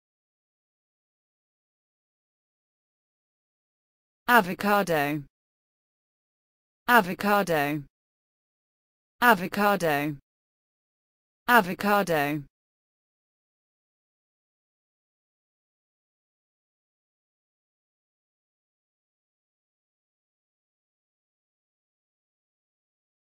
How to Pronounce avocado
How to pronounce avocado, this video will help you to pronounce it correctly